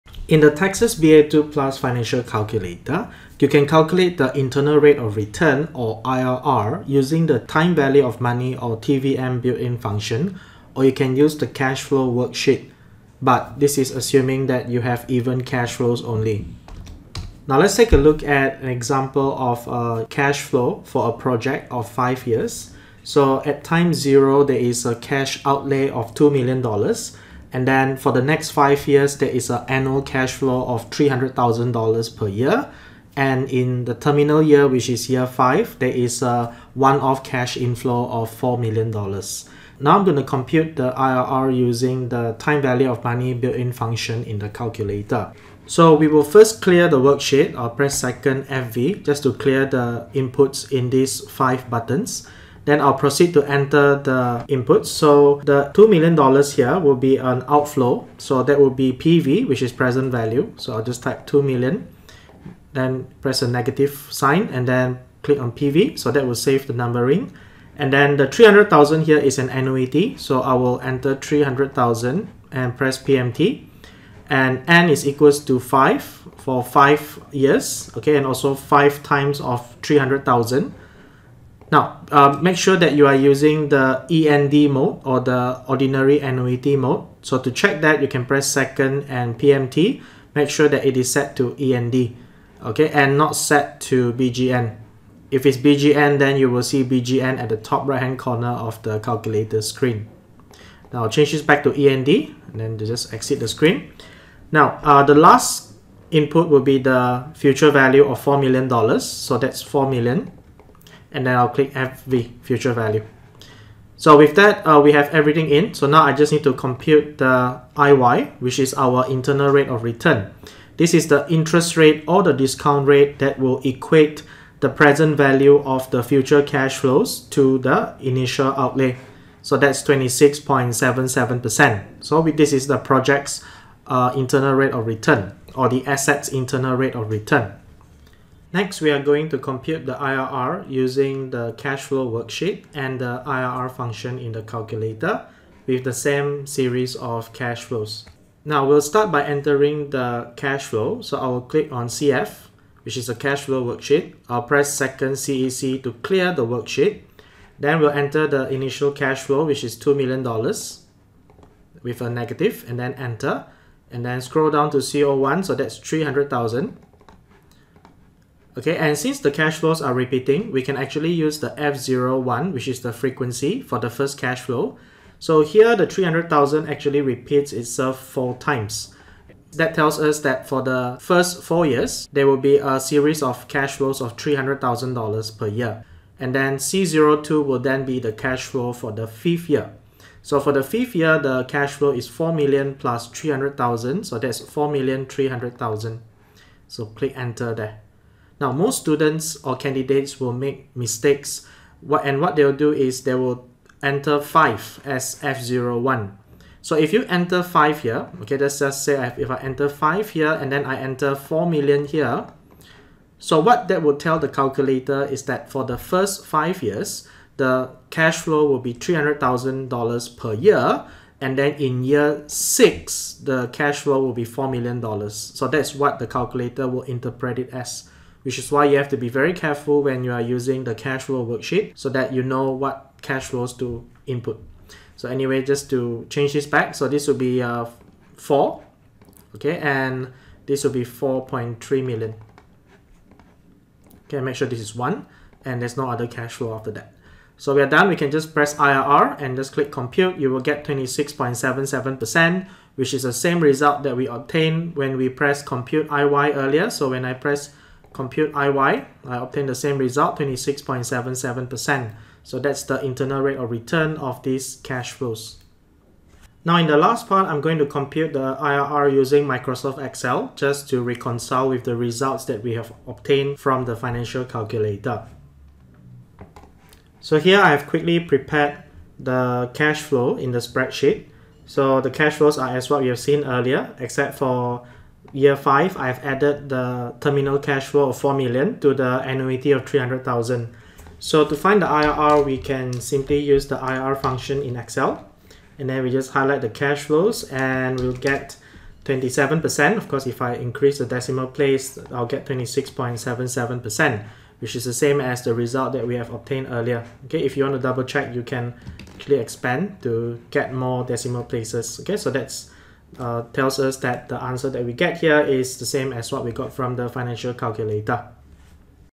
CFA Level 1 | Texas BA II Plus: Using TVM and CF to compute IRR
for more info on CFA prep courses in Malaysia, Singapore, or wherever you are.

This video post is in response to one comment on one of my videos on why I used TVM instead of the IRR function to compute the internal rate of return (IRR). This gave me the idea of doing a video just to provide further clarity for future viewers.

I will link the video where I got the comment from for further reference in the End Screen.

Thanks SIMFINSO.